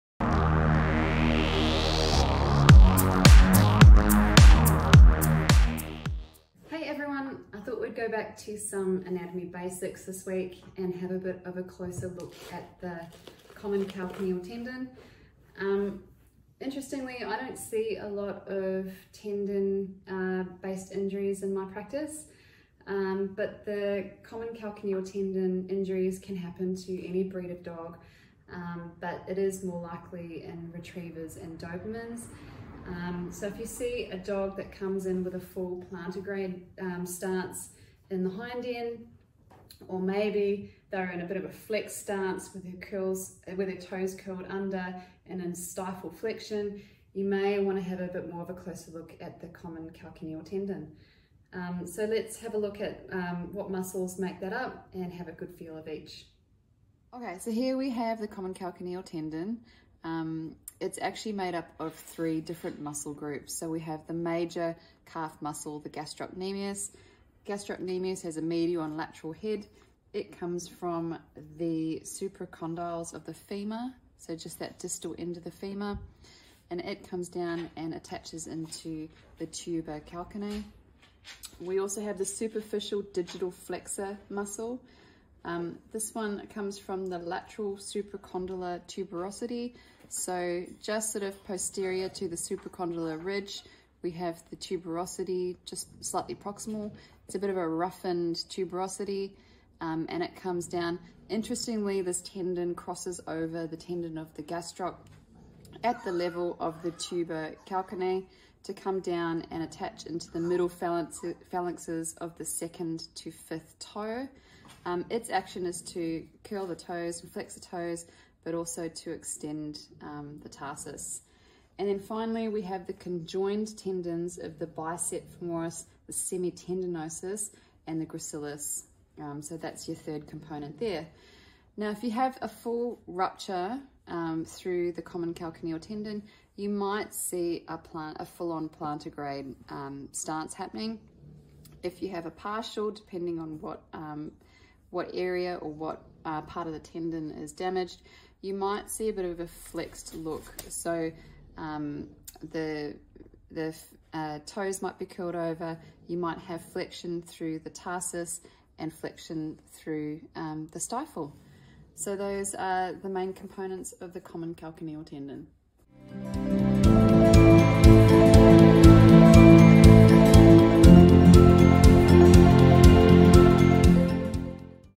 The anatomy of the common calcaneal tendon in dogs
Welcome to the Canine Kinetics YouTube channel, where we explore the world of canine rehabilitation.

In this video, we will demonstrate how to locate and palpate the common calcaneal tendon in dogs and look at the convergence of the three musculotendinous units that make up the common calcaneal tendon.

Remember, this is just a demonstration of proper technique and guidance is always recommended. Always consult a veterinary professional or canine physiotherapist before implementing new exercises, treatments or measurements.